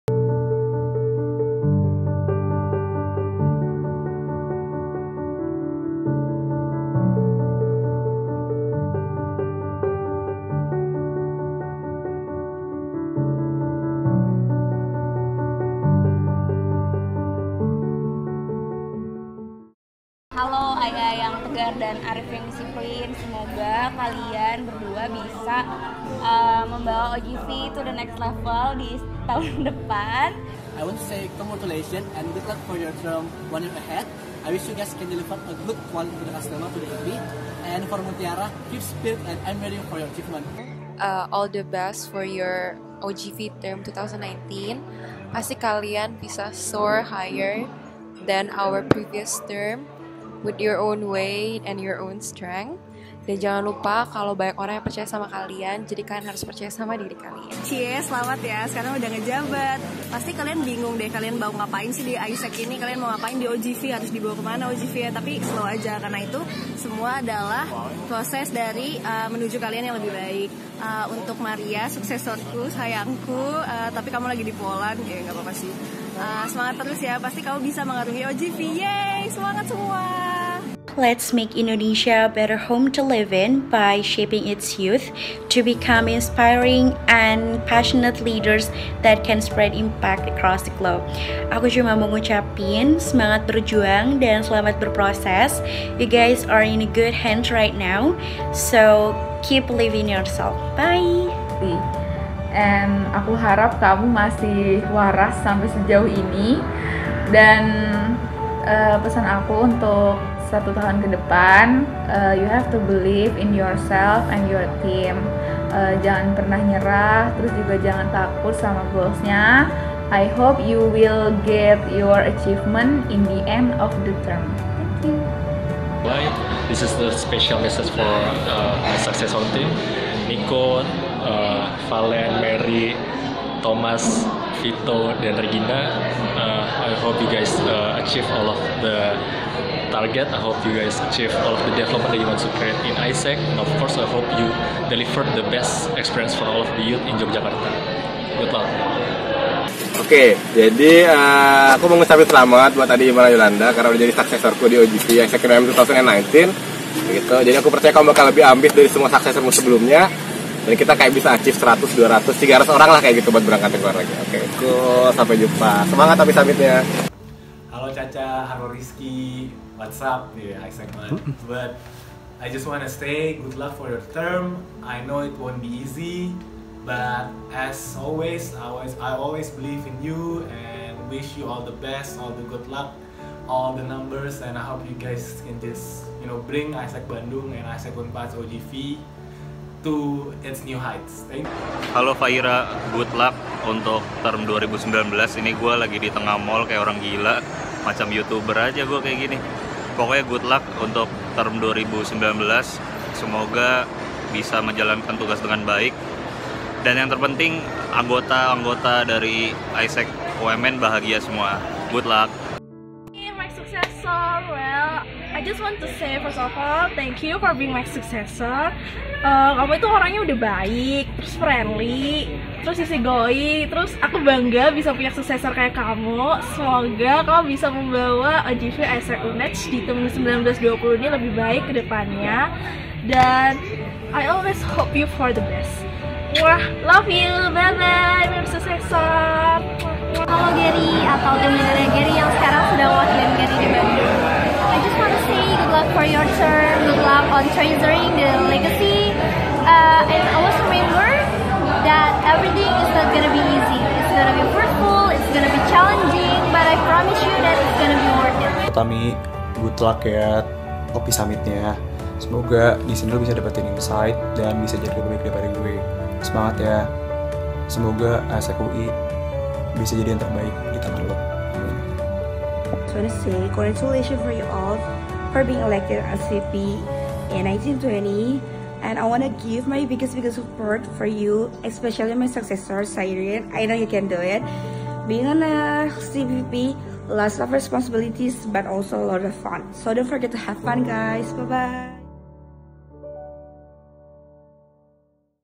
Achieveru to Wiraditya
The messages from oGV AIESEC in Indonesia term 2018 to oGV AIESEC in Indonesia term 2019. We as the previous generation believe to the new generation to carry on the legacy.